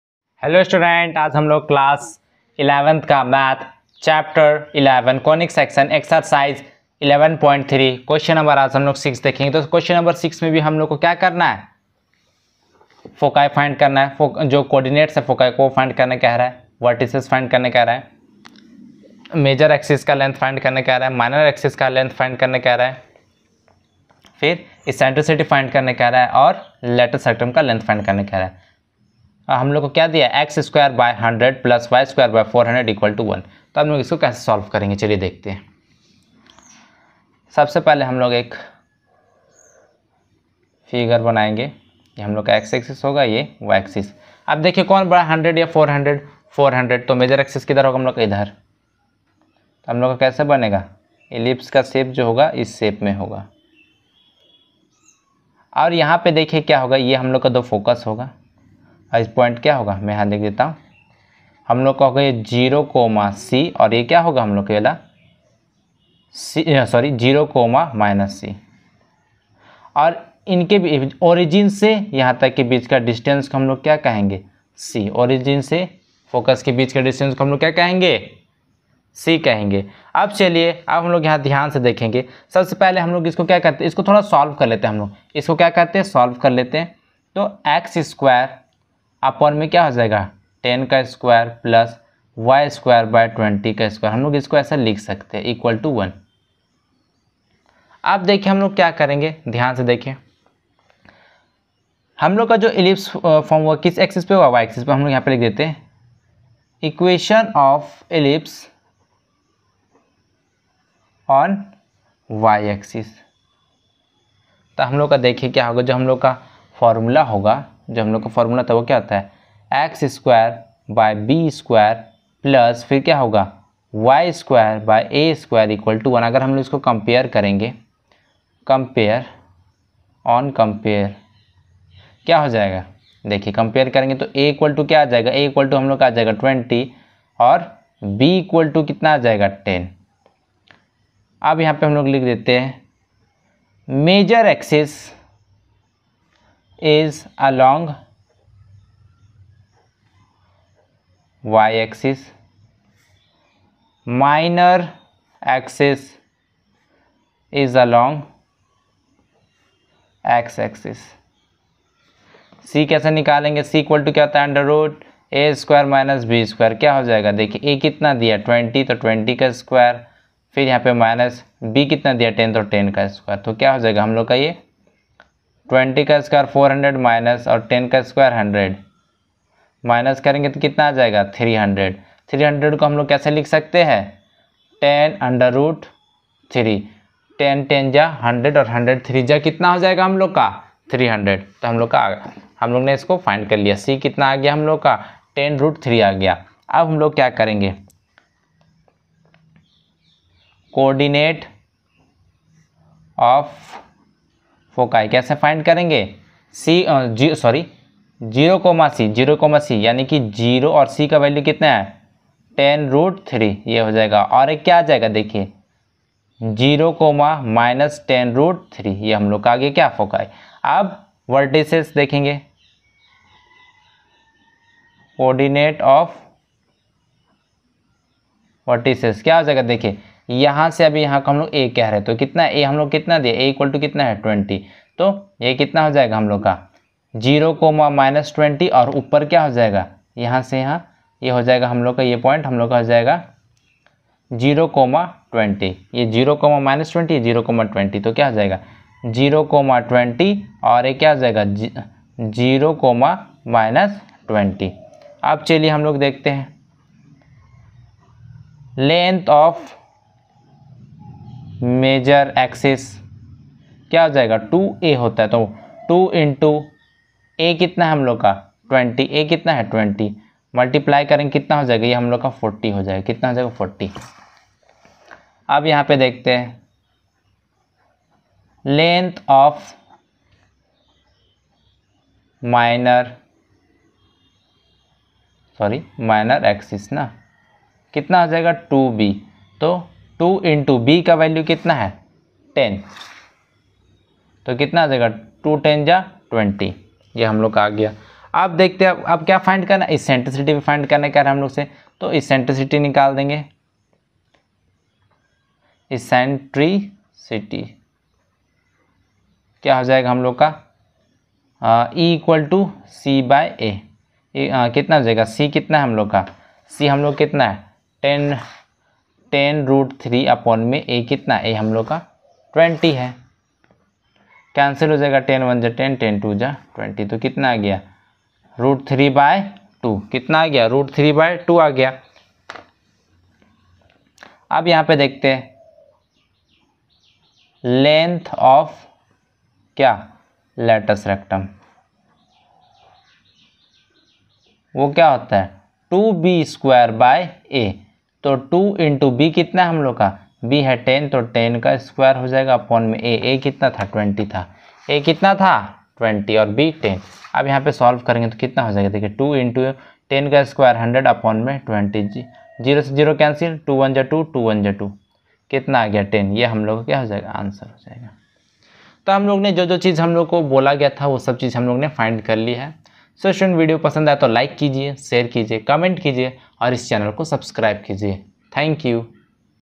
Class 11 || Chapter Conic Section Ex :- 11.3 || Question no.6 Answer.
Class 11 || Chapter Conic Section Ex :- 11.3 || Question no.6 Find the co-ordinates of the foci, the vertices, the length of major axis, the minor axis, the eccentricity and the length of the latus rectum of the ellipse.
(6) x²/100+y²/100=1